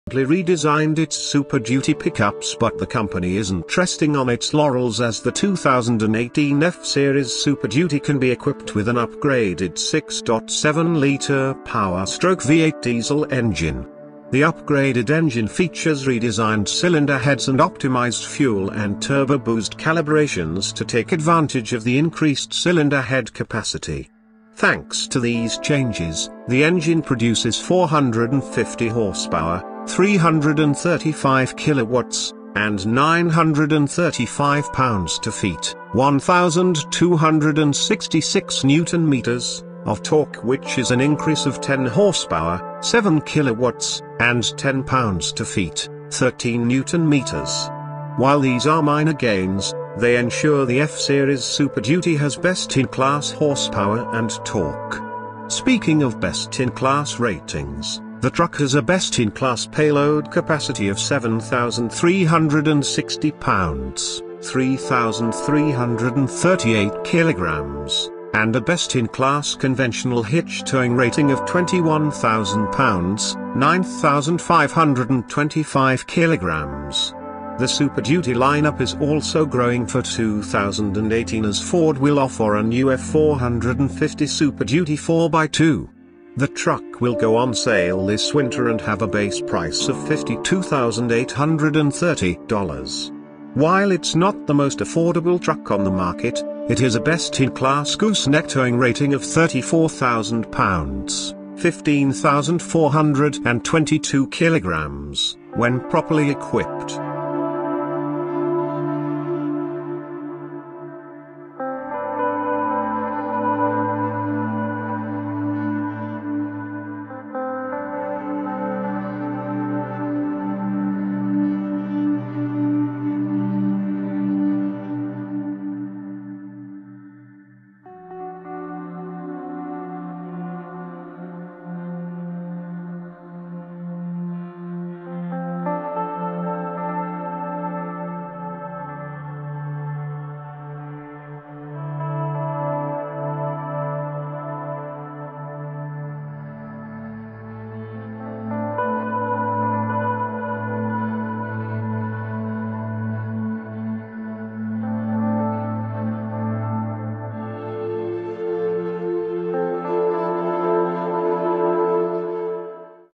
2018 Ford F-Series Super Duty Gains Upgraded Diesel Engine With 935 Lb-Ft Of Torque NEWS CAR 24H
2018 Ford F-Series Super Duty Gains Upgraded Diesel Engine With 935 Lb-Ft Of Torque